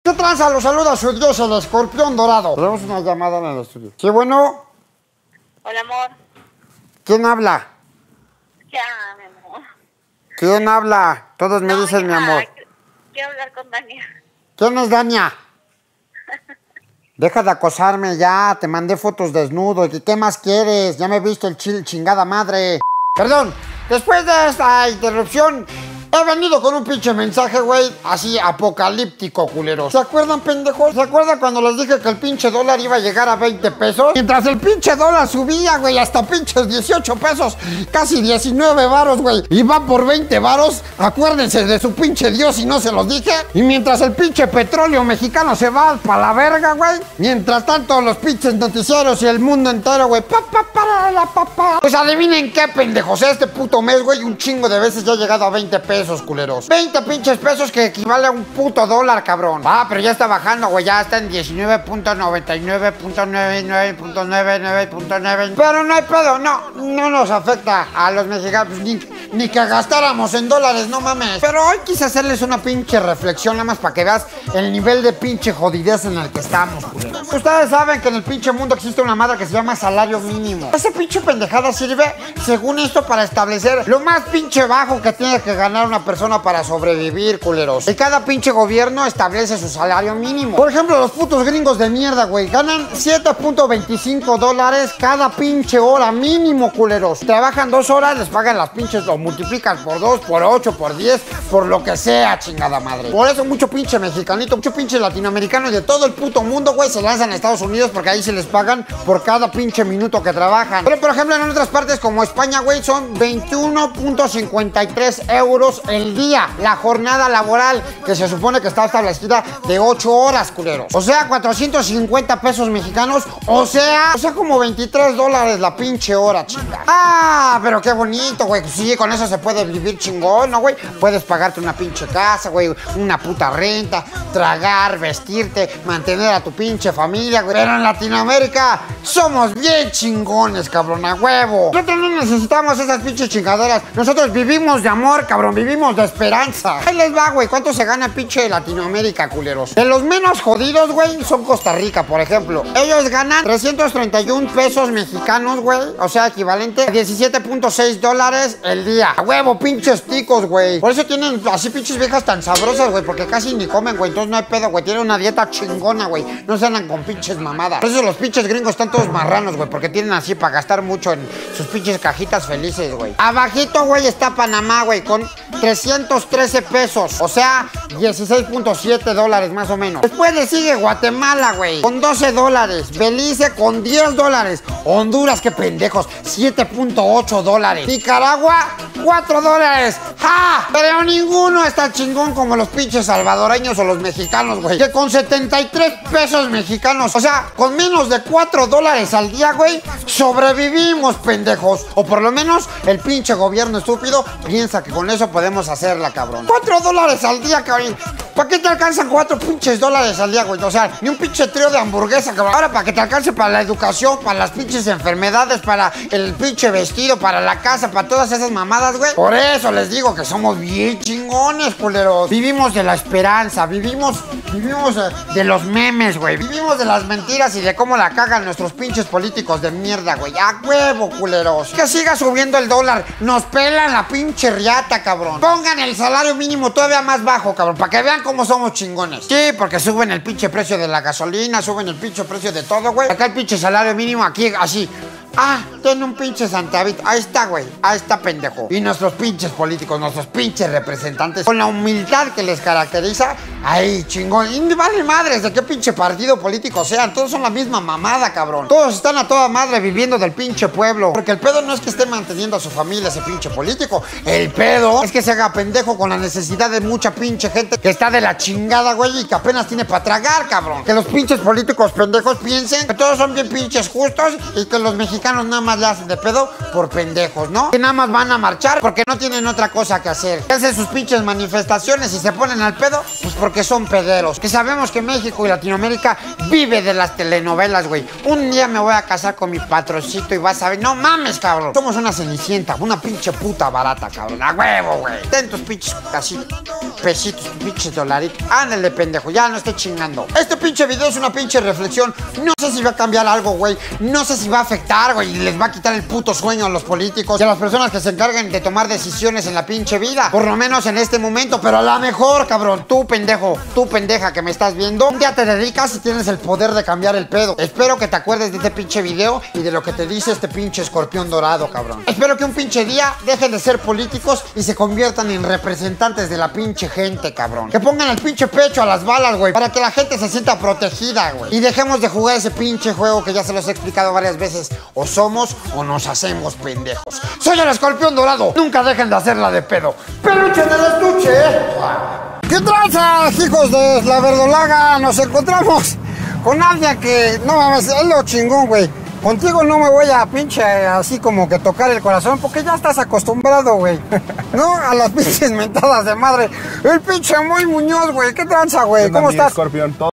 Los sueldos culeros de los mexicanos
¿Qué pasaría si vivieras en otro pinche país? ¡Con otro pinche salario mínimo de mierda!